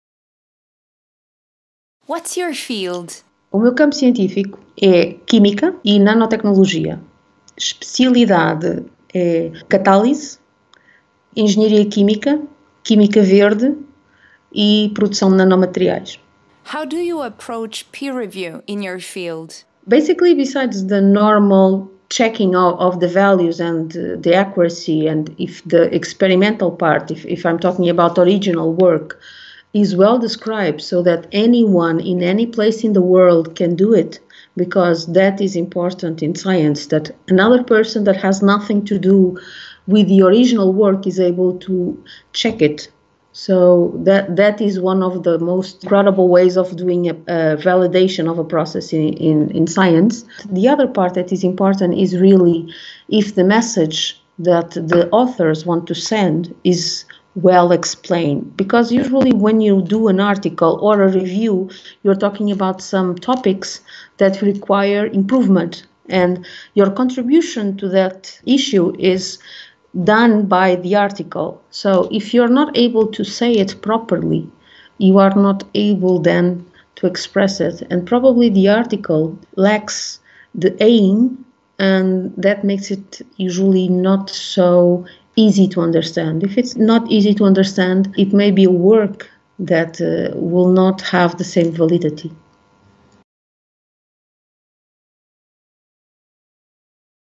Dissecting peer review with A.P. da Costa Ribeiro
This video is part of the teaching material I have produced for my project on peer review.

Transcript:
[Q] What's your field?
My field is chemistry and nanotechnology, in particular catalysis, chemical engineering, green chemistry, and the production of nanomaterials.
[Q] How do you approach peer review in your field?
Basically, besides the normal checking of the values and the accuracy and if the experimental part, if I'm talking about original work, is well described, so that anyone in any place in the world can do it. Because that is important in science, that another person that has nothing to do with the original work is able to check it. So that is one of the most credible ways of doing a validation of a process in science. The other part that is important is really if the message that the authors want to send is well explained, because usually when you do an article or a review, you're talking about some topics that require improvement and your contribution to that issue is done by the article. So, if you're not able to say it properly, you are not able then to express it, and probably the article lacks the aim and that makes it usually not so easy to understand. If it's not easy to understand, it may be work that will not have the same validity.